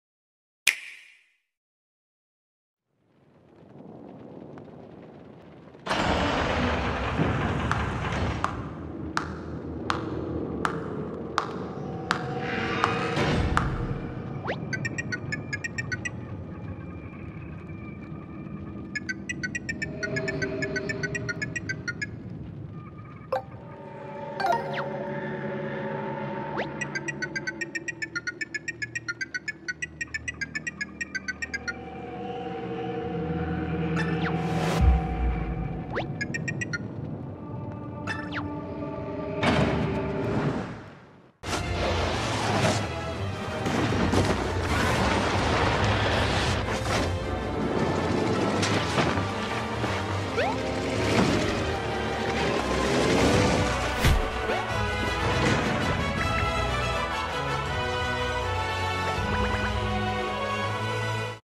Paper Mario Giveaway! Nintendo Switch 2020!
To celebrate the arival of Paper Mario on the Nintendo Switch, Mike Perez is giving away a copy of the game!  Winner  will be announced on July 10! But does Mike Perez has ever given away only one copy?